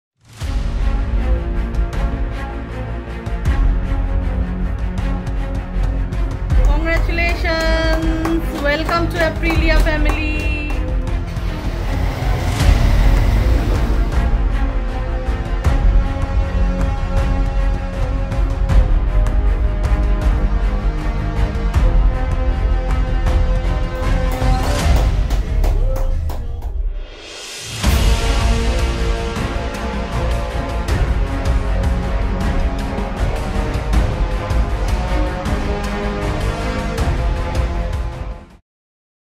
Delivery Ceremony of Mr. A RAHAMAN MONDAL from MOTOSHOP Apriliastore kolkata 🎉🛵🎊
📍𝗠𝗢𝗧𝗢𝗦𝗛𝗢𝗣 : 𝟱𝟯𝗖 𝗡𝗮𝗳𝗮𝗿 𝗖𝗵𝗮𝗻𝗱𝗿𝗮 𝗗𝗮𝘀 𝗥𝗼𝗮𝗱, 𝗝𝗮𝗺𝗲𝘀 𝗟𝗼𝗻𝗴 𝗦𝗮𝗿𝗮𝗻𝗶, 𝗡𝗲𝗮𝗿 𝗢𝗿𝗶𝗲𝗻𝘁 𝗗𝗮𝘆 𝗦𝗰𝗵𝗼𝗼𝗹 📲 𝟳𝟵𝟴𝟬𝟴𝟴𝟱𝟳𝟴𝟵 (𝘄)